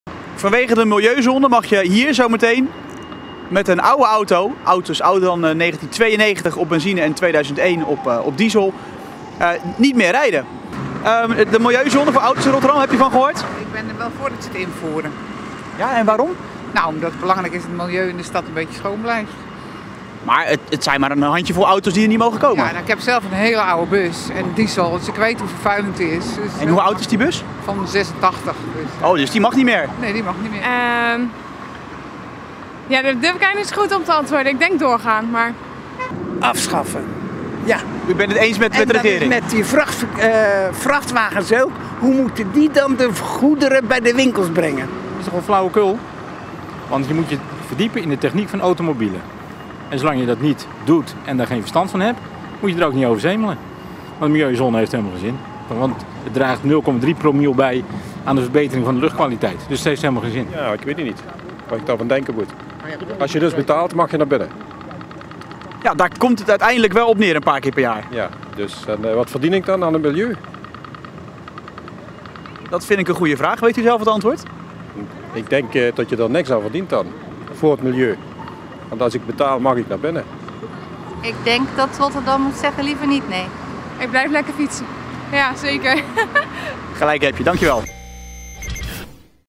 OPEN Vraag - Wat vind jij van de milieuzone?
Met de invoering van de Milieuzone op 1 januari moet de lucht in het centrum van Rotterdam schoner worden. Maar de Tweede Kamer steekt nu een stokje voor alle milieuzones voor personenauto’s.